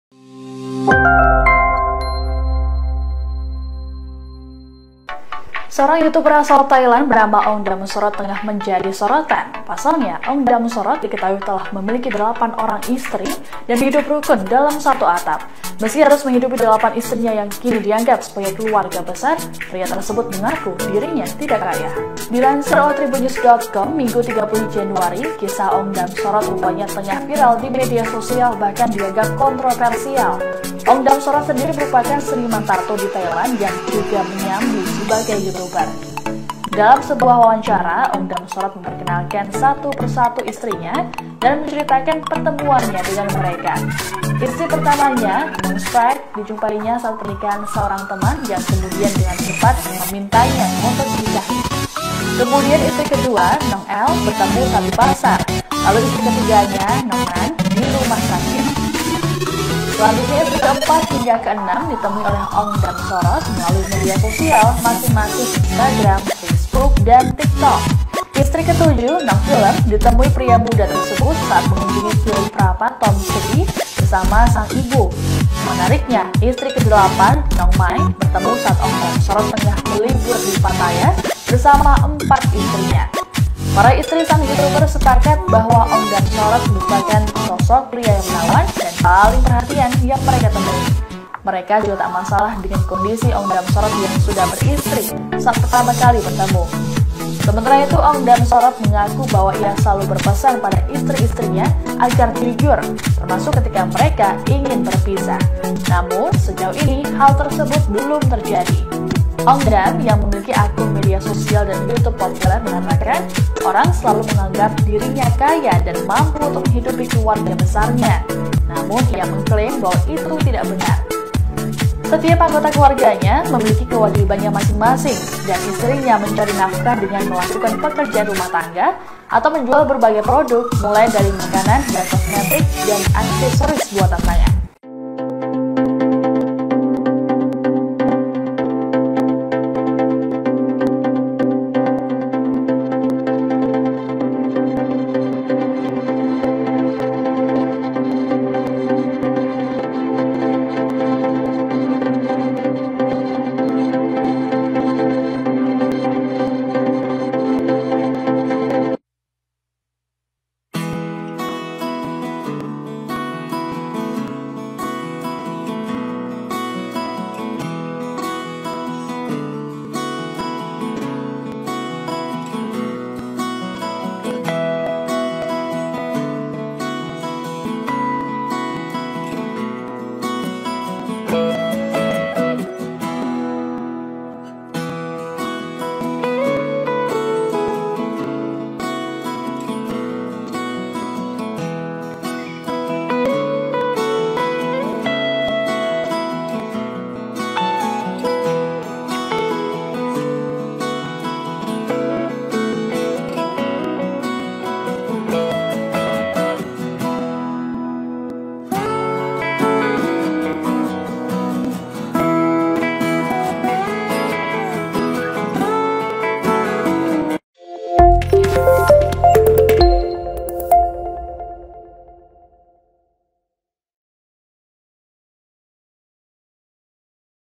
Pria Asal Thailand Ini Tidak Kaya, Tapi Punya Istri Banyak
Seorang YouTuber asal Thailand bernama Ong Dam Sorot tengah menjadi sorotan.

Pasalnya, Ong Dam Sorot diketahui telah memiliki delapan orang istri dan hidup rukun dalam satu atap.

Meski harus menghidupi delapan istrinya yang kini dianggap sebagai keluarga besar, pria tersebut mengaku dirinya tidak kaya.

Ong Dam Sorot sendiri merupakan seniman tato di Thailand yang juga menyambi sebagai YouTuber.

Dalam sebuah wawancara, Ong Dam Sorot memperkenalkan satu per satu istrinya dan menceritakan pertemuannya dengan mereka.

Istri pertamanya, Nong Sprite dijumpainya saat pernikahan seorang teman yang kemudian dengan cepat memintanya untuk dinikahi.

Kemudian istri kedua, Nong L bertemu saat di pasar, lalu istri ketiganya, Nong Nan di rumah sakit.

Istri ketujuh, Nong Film, ditemui pria muda tersebut saat mengunjungi kuil Phra Pathom Cedhi bersama sang ibu.

Menariknya, istri ke-delapan, Nong Mai bertemu saat Ong Dam Sorot tengah berlibur di Pattaya bersama empat istrinya.

Para istri sang YouTuber sepakat bahwa Ong Dam Sorot merupakan sosok pria yang menawan dan paling perhatian yang mereka temui.

“Dia mungkin pria paling perhatian yang pernah kami temui,” ujar seorang istrinya.

Mereka juga tak masalah dengan kondisi Ong Dam Sorot yang sudah beristri saat pertama kali bertemu.

Sementara itu, Ong Dam Sorot mengaku bahwa ia selalu berpesan pada istri-istrinya agar jujur, termasuk ketika mereka ingin berpisah.

Namun sejauh ini, hal tersebut belum terjadi.

Setiap anggota keluarganya memiliki kewajibannya masing-masing, dan istrinya mencari nafkah dengan melakukan pekerjaan rumah tangga, atau menjual berbagai produk, mulai dari makanan hingga kosmetik dan aksesoris buatan tangan.

Sumber : Tribun Batam